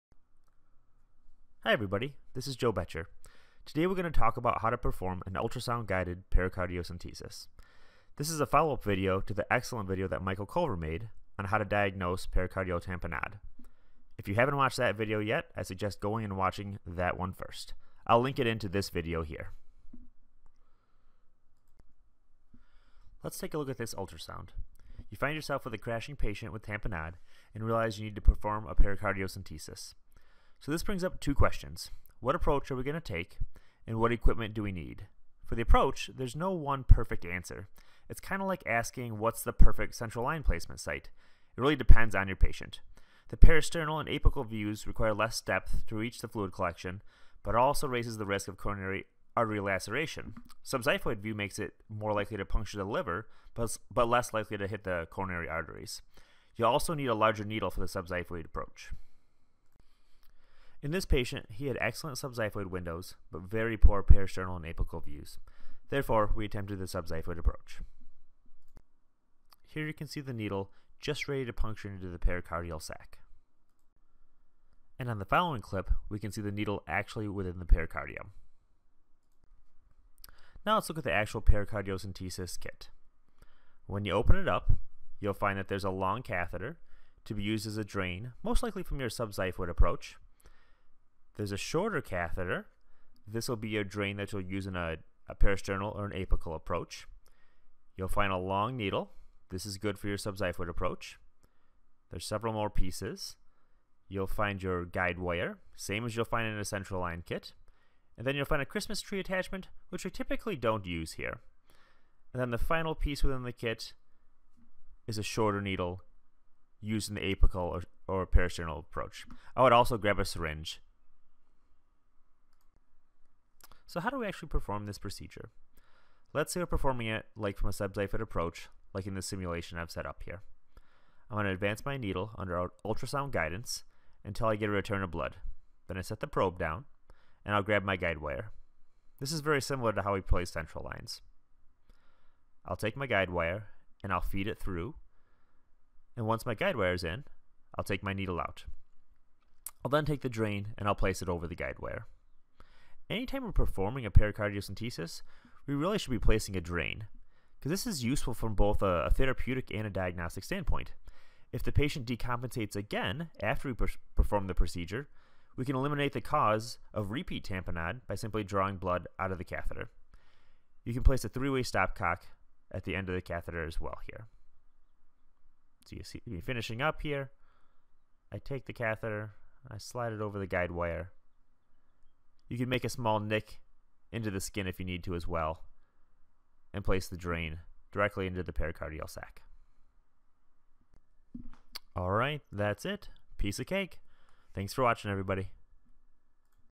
Ultrasound Guided Pericardiocentsis and Drain Placement
How to perform a ultrasound guided pericardiocentesis, and the equipment you'll need to place a drain along with this.